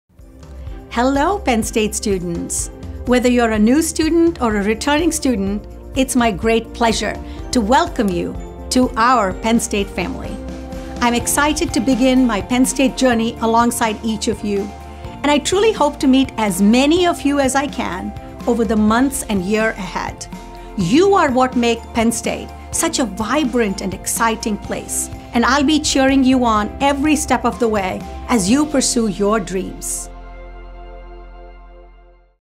Neeli Bendapudi — Welcome to new and returning Penn State students
As the University’s second session of summer classes kicks off, Penn State President Neeli Bendapudi shares a message with new and returning students.